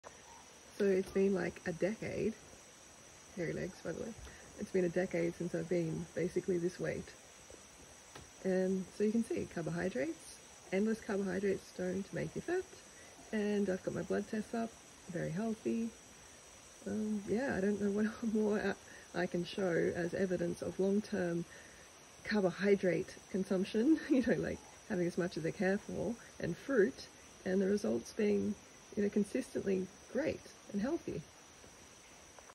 15 year vegan results. the deterioration is real. Send help!
i’ve been vegan eating unlimited fruit and carbohydrates for 15 years! my diet can be found